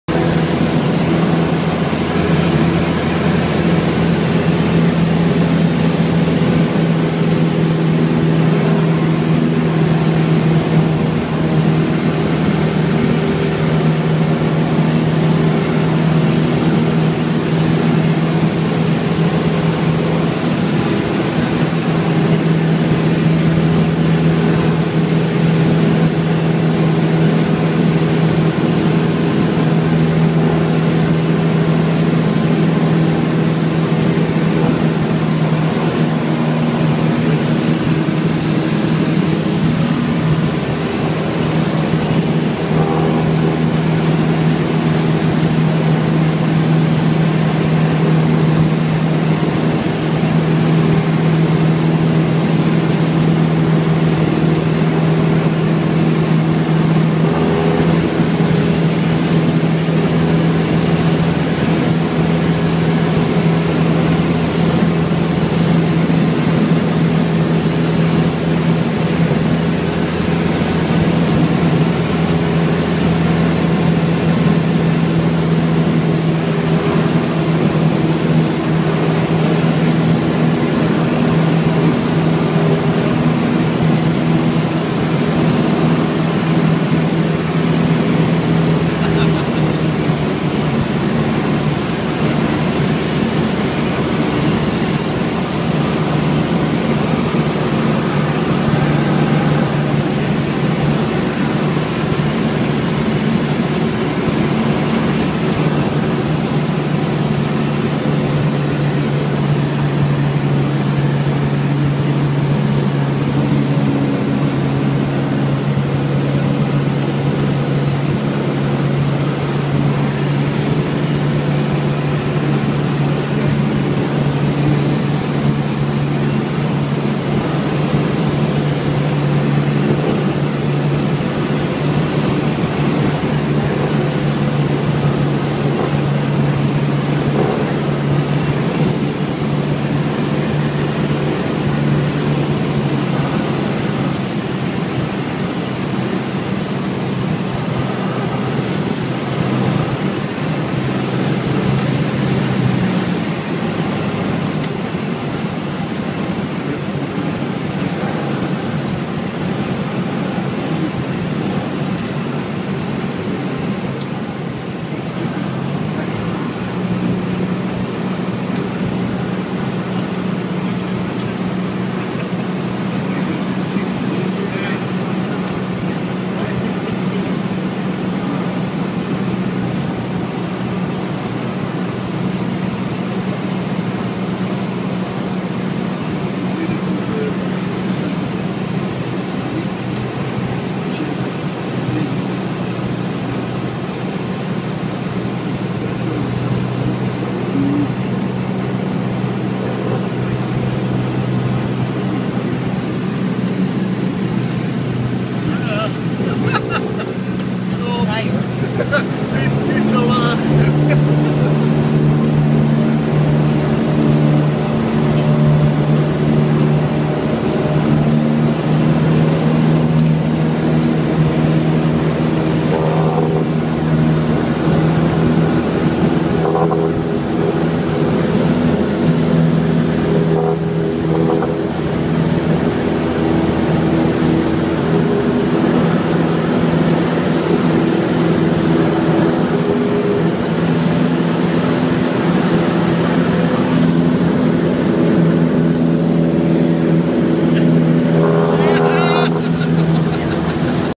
Joran Buwalda in another C-172
Joran Buwalda (15)  returns to Ueterson airfield for another flight with aviation veteran Rob Helmers. 4-1-2013
Thank you Air Hamburg!